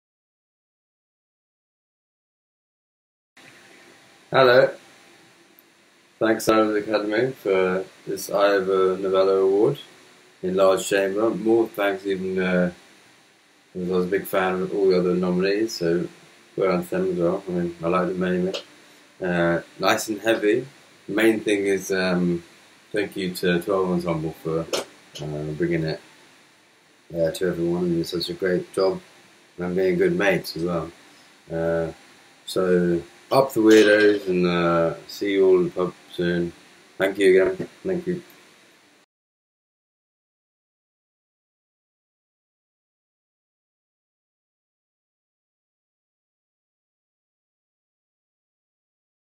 Winner of Large Chamber, Oliver Leith tells us what receiving the Ivor Novello Award means to him
Watch Oliver Leith accept the Ivor Novello Award for Large Chamber at The Ivors Composer Awards 2020.

Ivor Novello Award winners across fourteen categories were revealed live on BBC Radio 3 on 1 December. Listen to the original ceremony broadcast on BBC Radio 3